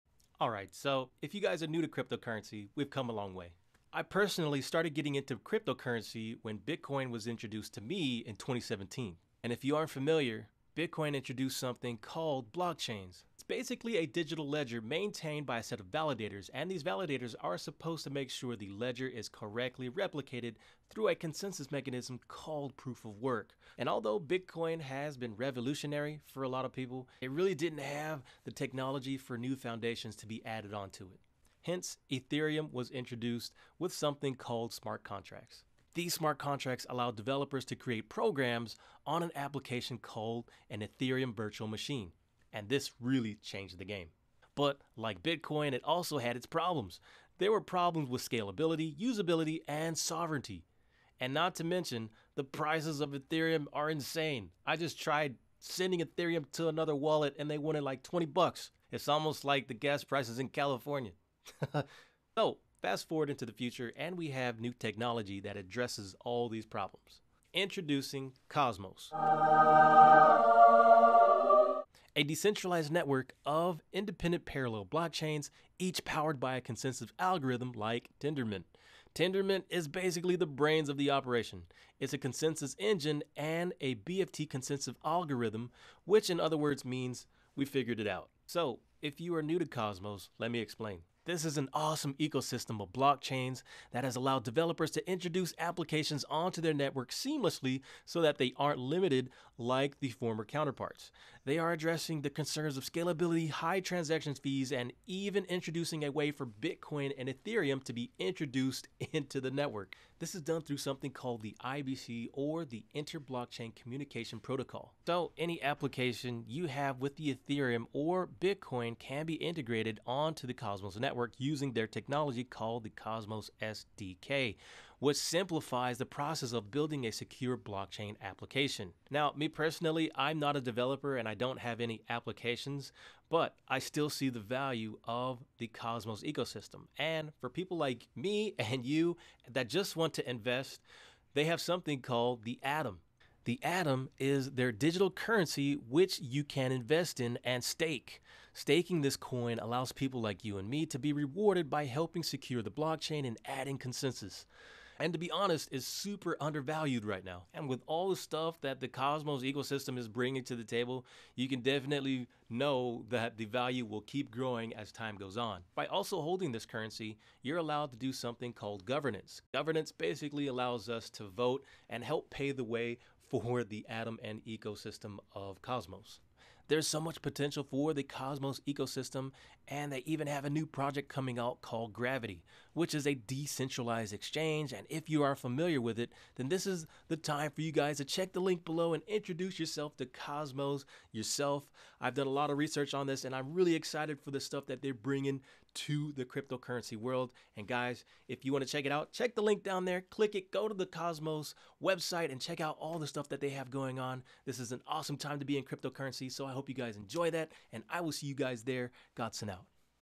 Discover Cosmos Hub & $ATOM - the heart of the Internet of Blockchains
cosmos atom is a new blockchain crypto currency that allows people to avoid the problems of high gas fees and scalability in the ecosystem.
today i will be introducing bitcoin, Ethereum and cosmos (atom) and explain which one is the best and how you can profit from investing in them.